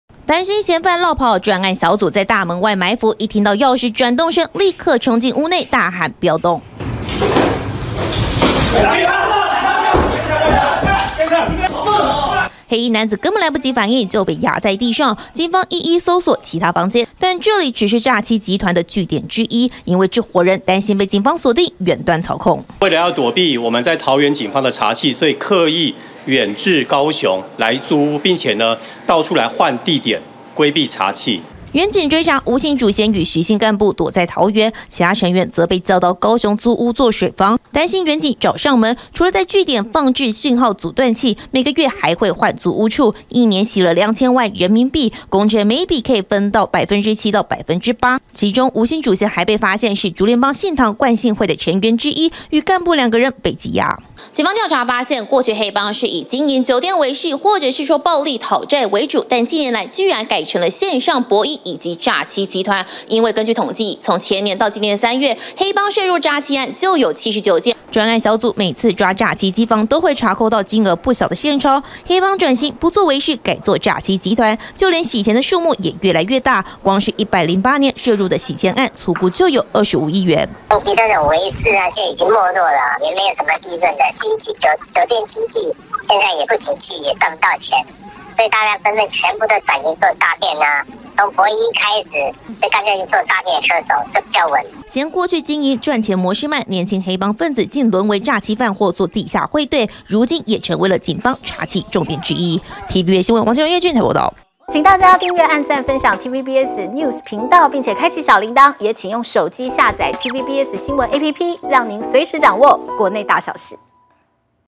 黑幫轉型! 涉地下匯兌.詐欺.博奕 1年洗錢逾25億｜TVBS新聞
刑事局破獲詐騙集團，沒想到再度逮捕黑幫分子！
主嫌找同學幫忙洗錢，為了躲避追查，自己和好友躲在桃園，叫底下成員到高雄租屋做水房，一年來經手人民幣兩千萬，但根據警方統計，黑幫轉型利用詐欺集團賺錢，光是前年到今年三月就破獲79件相關案件，粗估一年洗錢金額高達25億元。